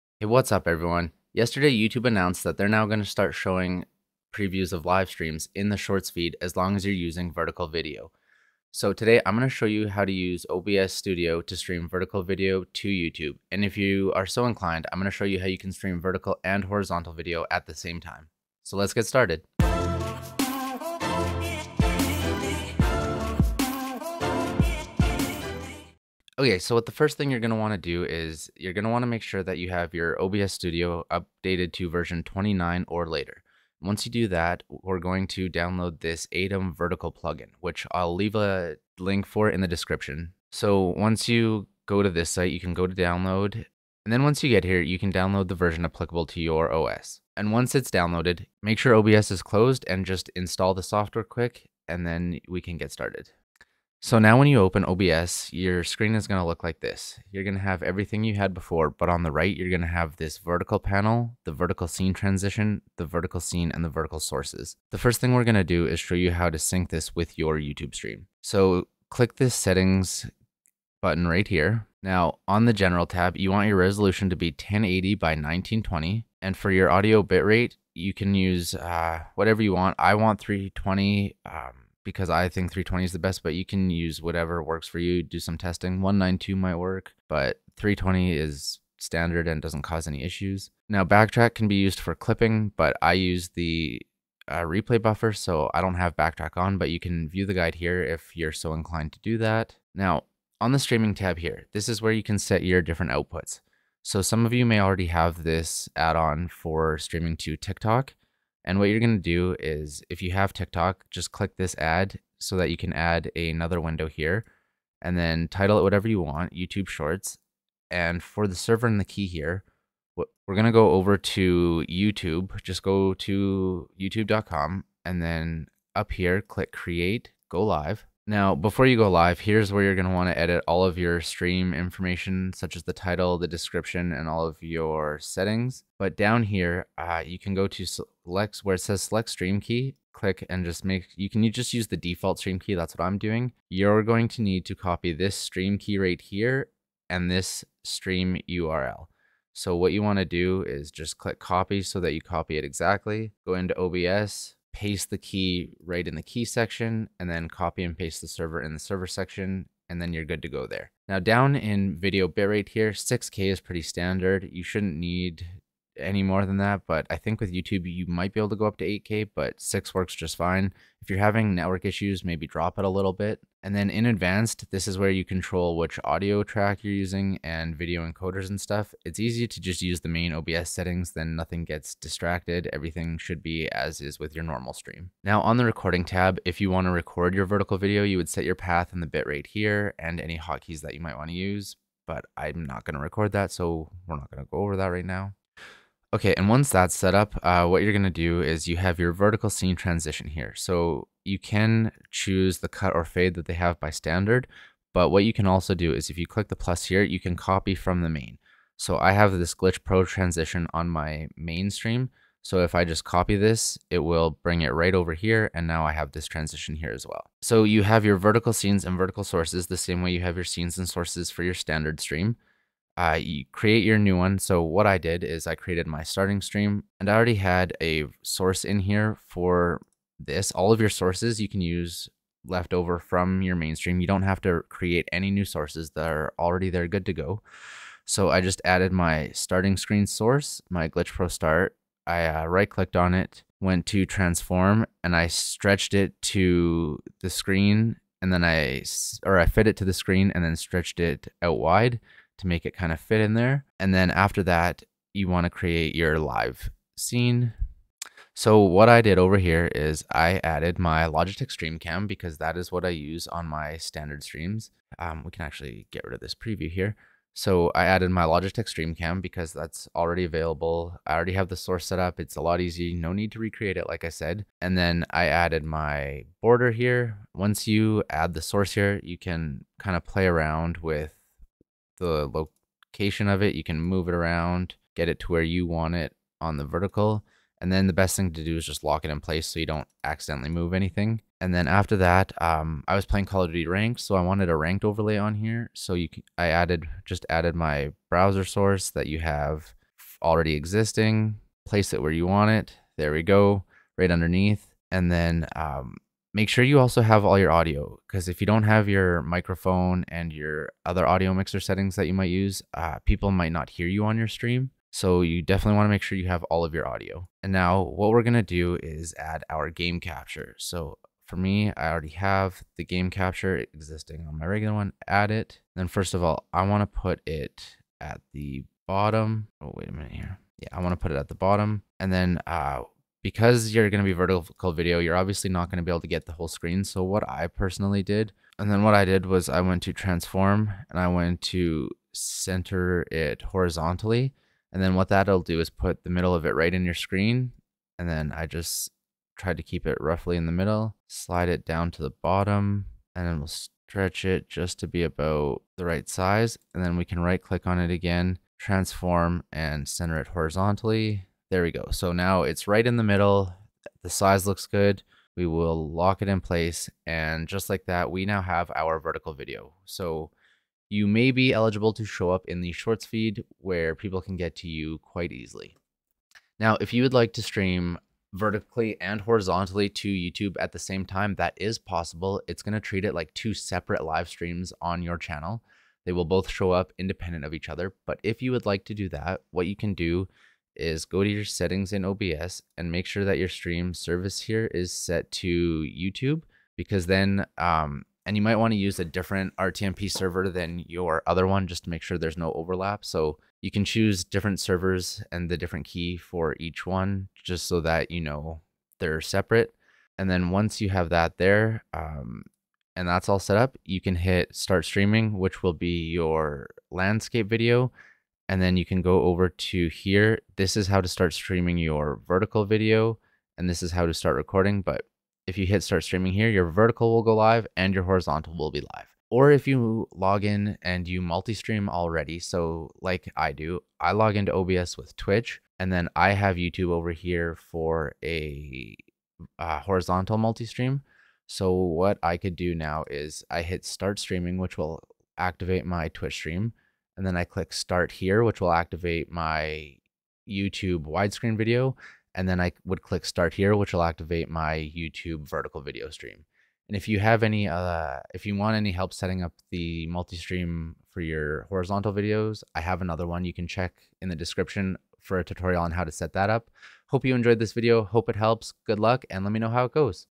Stream VERTICAL from OBS to YouTube Shorts! (FREE)
YouTube has recently announced that qualifying live videos will have previews show up in the Shorts feed that links directly to your stream! Check out this easy guide on how to setup vertical video streaming from OBS Studio to YouTube for your chance to get featured. I will also show you how landscape and vertical videos can be broadcast to YouTube simultaneously!

0:01 - Intro
0:26 - Plugin Install
0:53 - OBS Setup
2:02 - YouTube Key & RTMP
3:16 - Scene Transition
3:38 - Scenes
3:48 - Sources Setup
6:16 - Landscape & Vertical
7:36 - Outtro